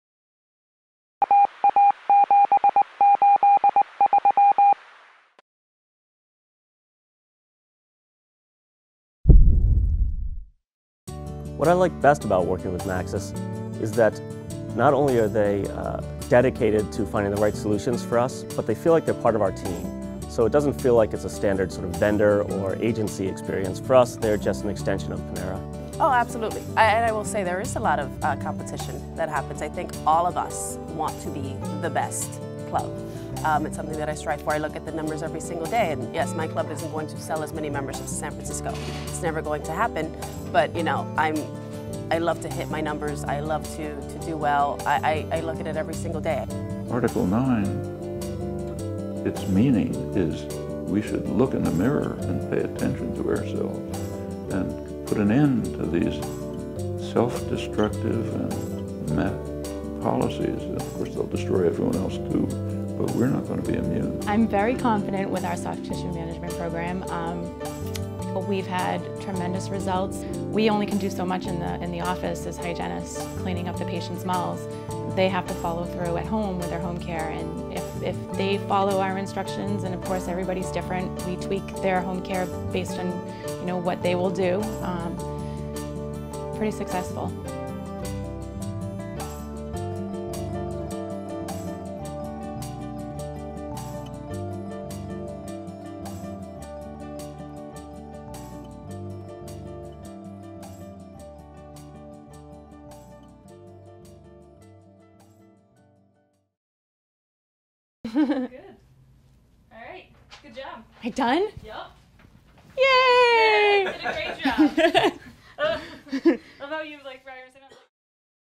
Sha La La La Productions: Interview & Testimonial Demo video| Boston Video Production
A new demo reel highlighting interview and testimonial videos that will help you stand out!

Need to produce interviews, testimonial or promotional video for your venue or company in Boston, MA? for more information.